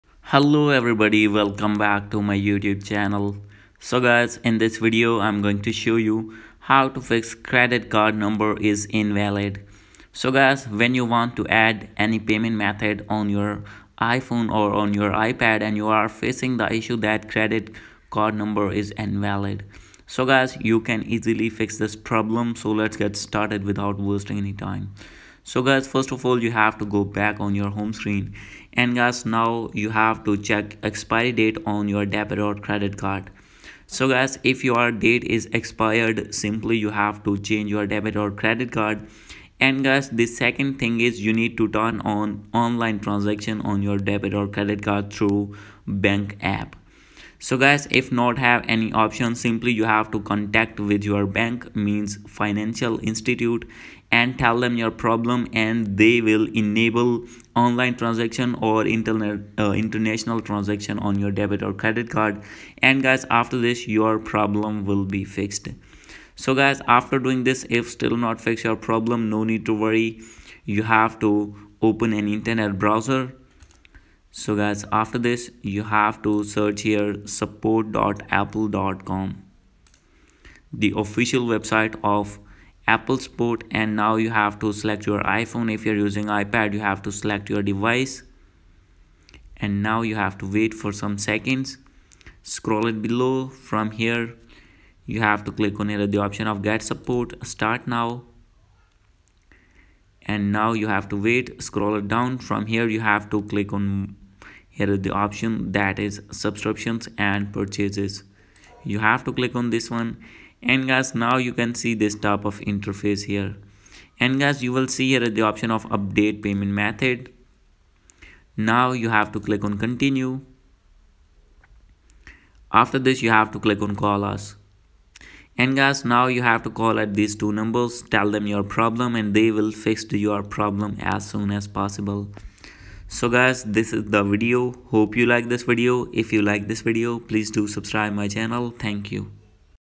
How to Fix Credit Card Number is Invalid on iPhone/iPad
In this video, I will help you to fix Credit Card Number is Invalid on iPhone. Please watch till the end.

Comments